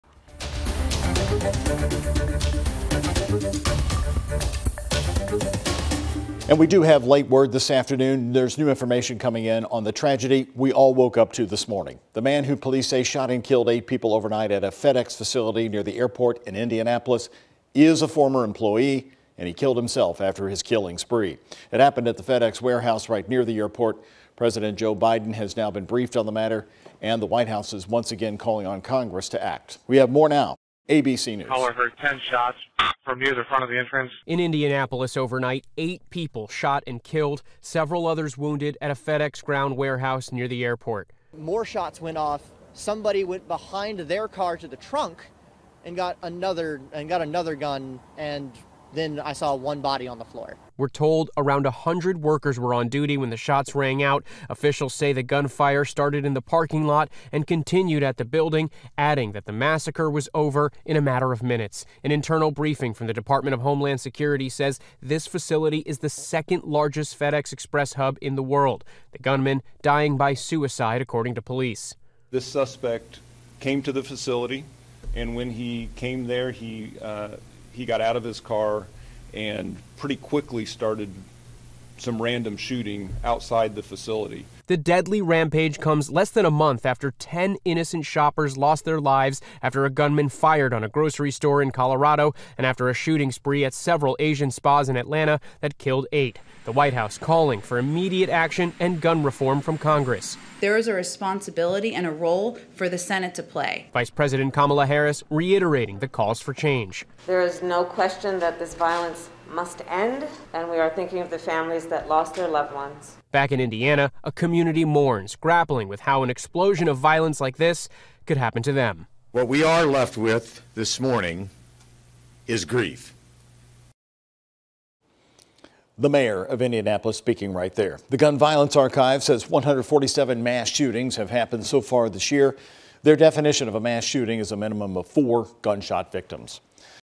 Shooter who killed 8 people at FedEx facility in Indianapolis was former employee
In Indianapolis, 8 people were killed and several others wounded during a mass shooting at a FedEx ground warehouse.